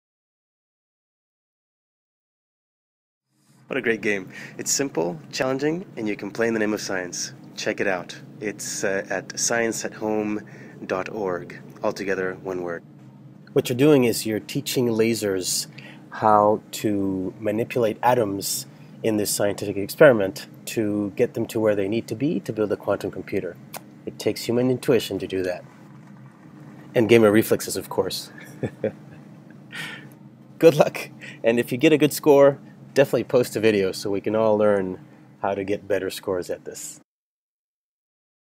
Playing for Science - Gamers help Quantum Computers
What a GREAT GAME... Simple, easy-to-learn and for a good cause. Hey, you're playing in the name of science! =)
More info at this link below including videos with tips about getting better scores and the science of Quantum Computers and lasers that you're playing to improve...